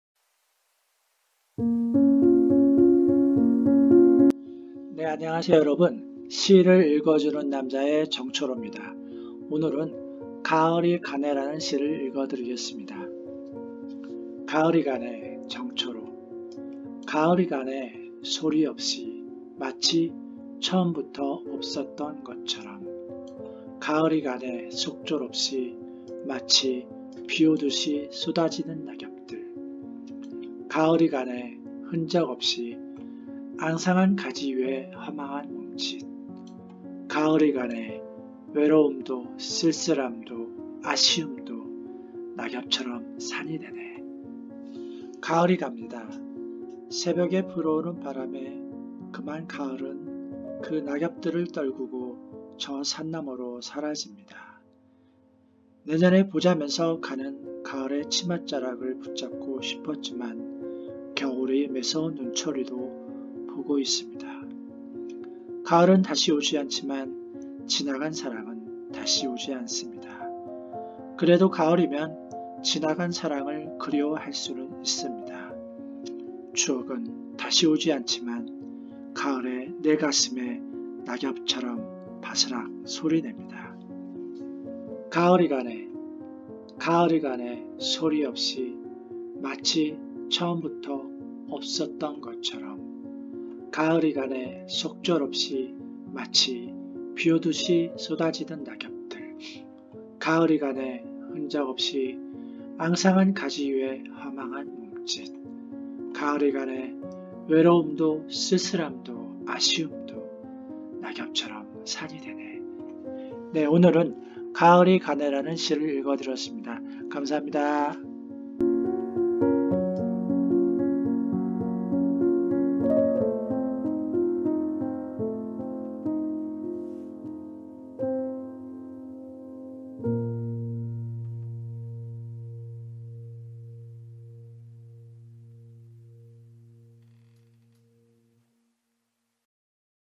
가을이가네-시낭송
가을이 갑니다. 새벽에 불어오는 바람에 그만 가을은 그 낙엽들을 떨구고 저 산너머로 사라집니다. 내년에 보자면서 가는 가을의 치맛자락을 붙잡고 싶었지만 겨울이 매서운 눈초리로 보고 있습니다. 가을은 다시 오지만 지나간 사랑은 다시 오지 않습니다. 그래도 가을이면 지나간 사랑을 그리워할 수는 있습니다. 추억은 다시 오지 않지만 가을에 내 가슴에 낙엽처럼 바스락 소리냅니다.